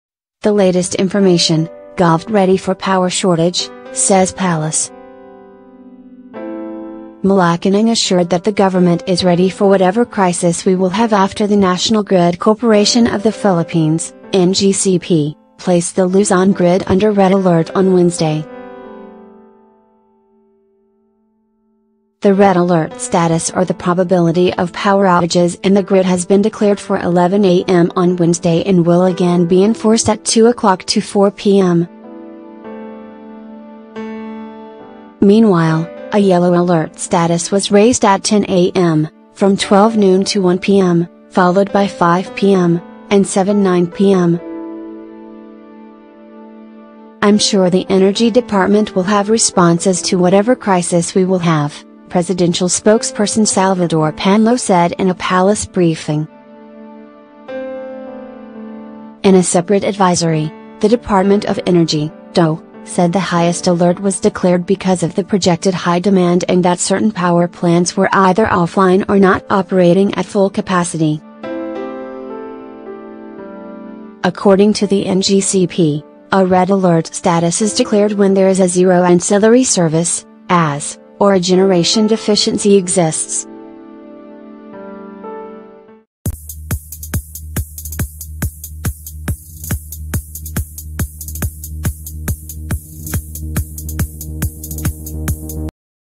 [The Latest Information] Gov’t ready for power shortage, says Palace
Malacañang assured that the government is ready for “whatever crisis we will have” after the National Grid Corporation of the Philippines (NGCP) placed the Luzon grid under red alert on Wednesday.